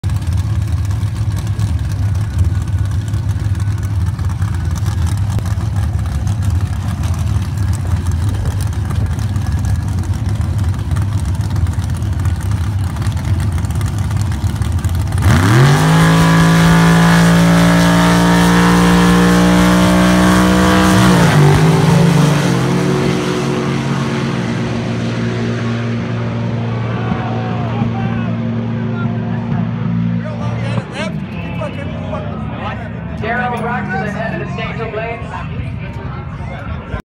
Brett Wagner (the big swag) run in gimli mb
Brett Wagner run in gimli mb Canada July 31 2016 Interlakenhoheweg dragway